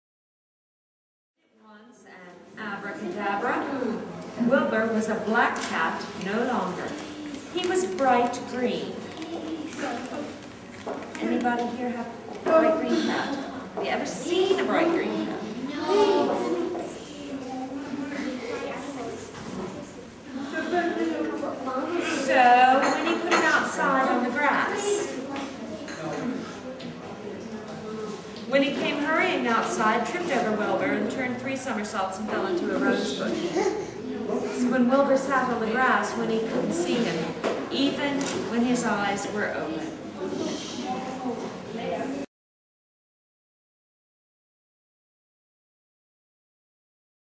Spirit of Voice Storytelling
Spirit of Voice festival presents a storytelling event at Galway City Museum, Ireland on Friday 31 October 2008. The Halloween themed stories told courtesy of Mary Judy from Dubray books.  This event took place prior to the launch of Telling Tales audiovisual storytelling DVD launch accessible for Deaf people.